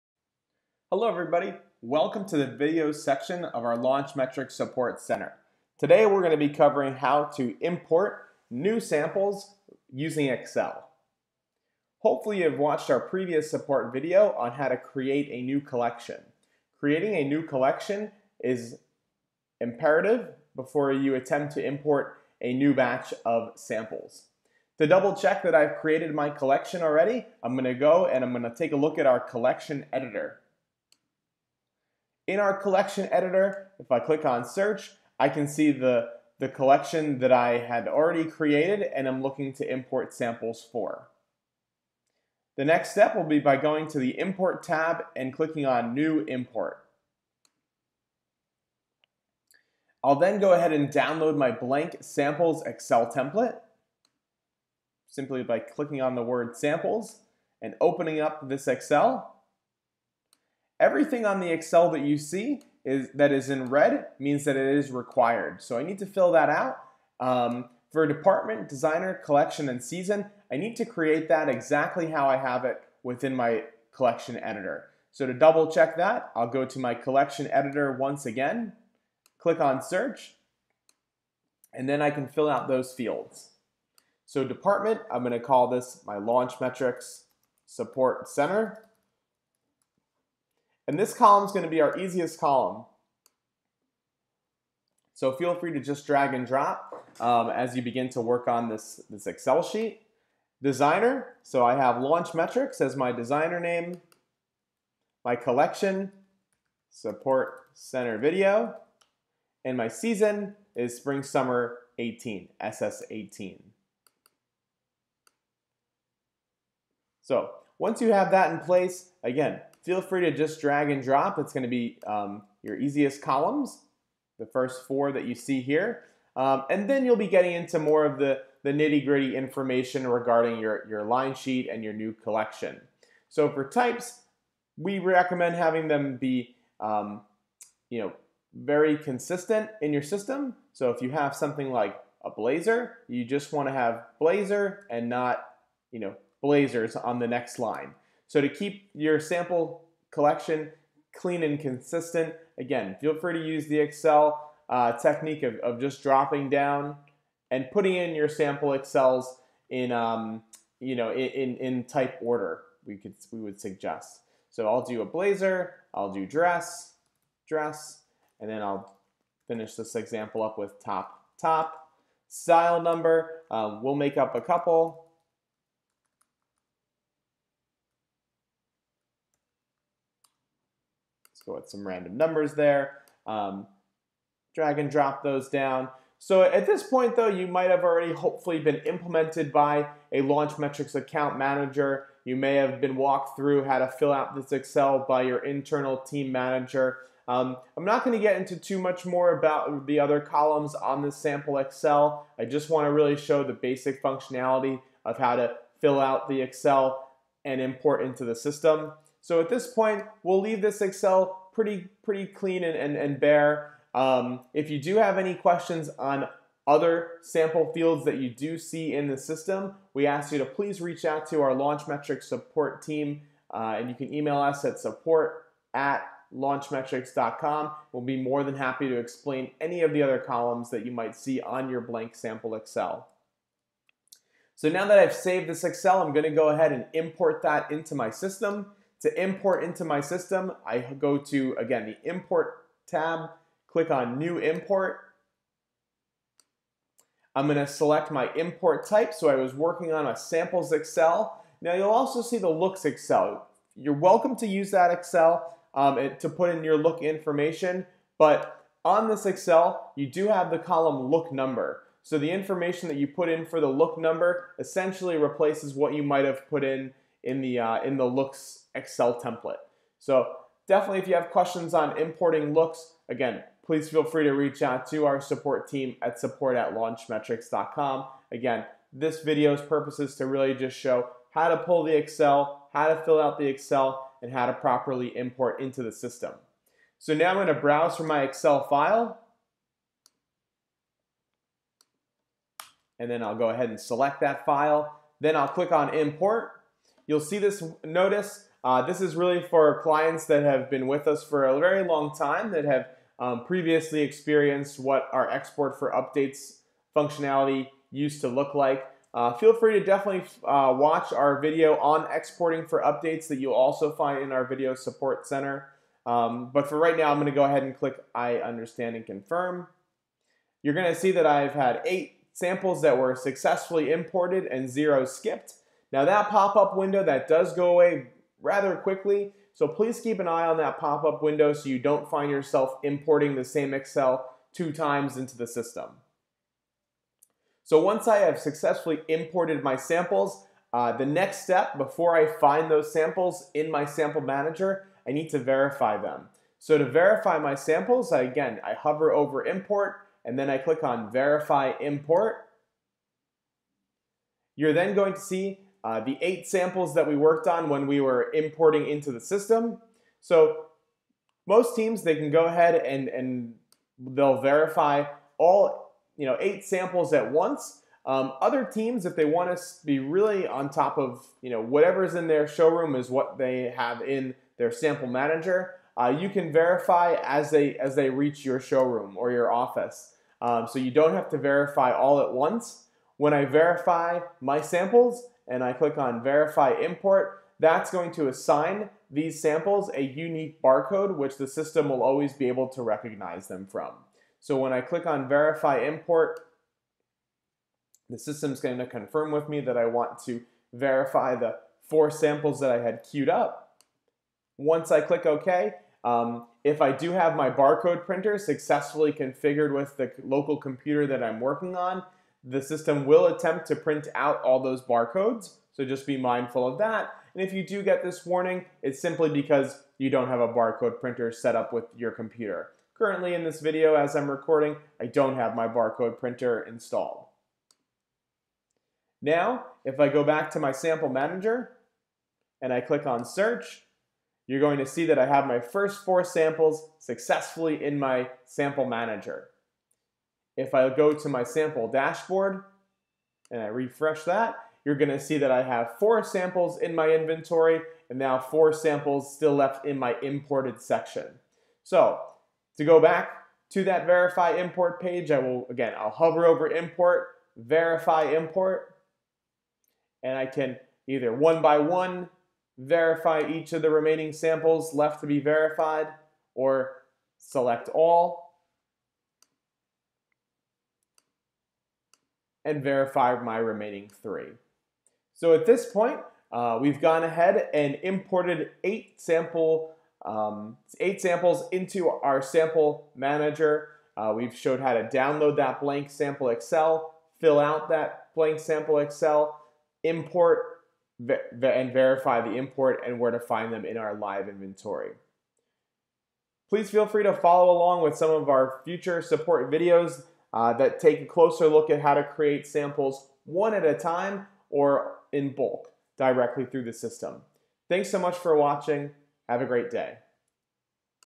Launchmetrics - Creating Multiple Samples via Excel
This Launchmetrics support video explains how to create multiple Samples by importing using a blank samples excel. For more information please visit these support articles: